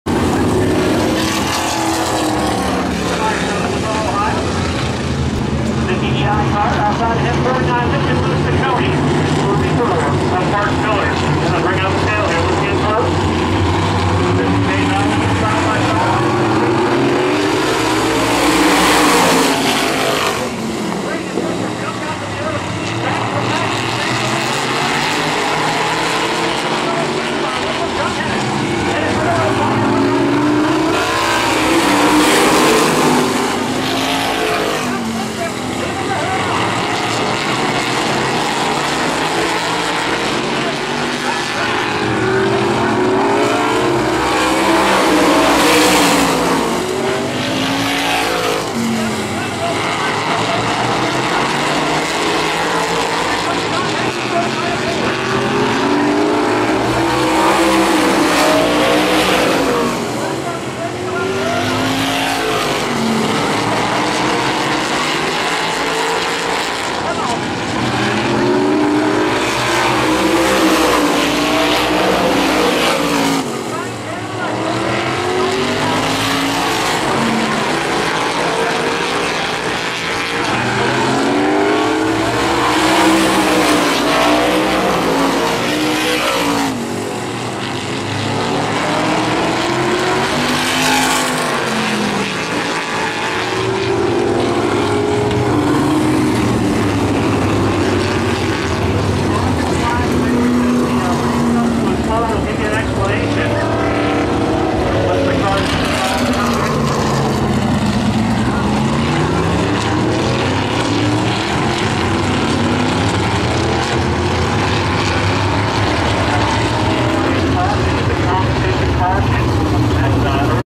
MSA Supermodifieds Lorain County Speedway Heat 2 07.16.2011
Wing Side Up Enterprises video of heat race two for the Midwest Supermodified Association presented by Town Money Saver at Lorain County Speedway. Held 07.16.11.  is winner. Video shot by Danette Ray

To learn more about this event, sanctioning body and "The Ultimate Short Track Race Car" where it's "All Supermodifieds, All of the Time!"